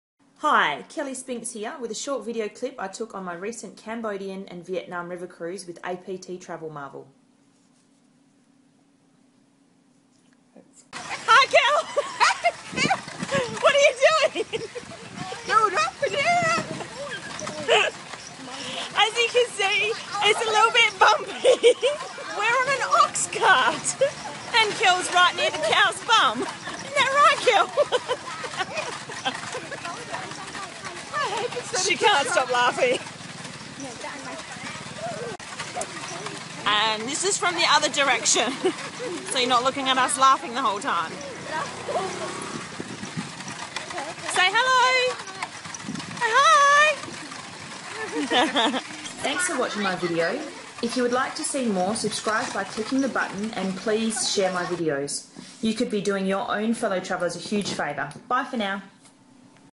Giggling all the way...
Kelly and Kellee having a hilarious time on an oxcart ride on the way to Oudong, Cambodia. This is another of the daily excursions on their Cambodian and Vietnam river cruise along the Mekong river.

Your Travel Planners
151 Albany Highway, Albany, WA 6330, Australia